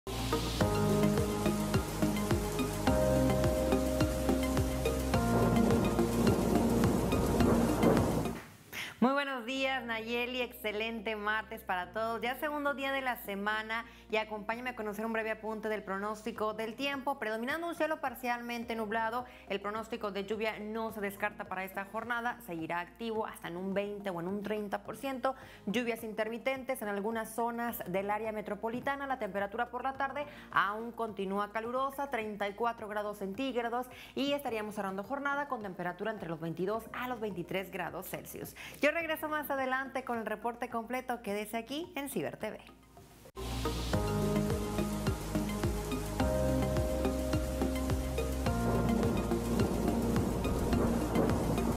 AVANCE CLIMA 17 AGOSTO YULISSA CASTILLO
Yulissa Castillo nos ta el avance del Clima de Monterrey y su área metropolitana.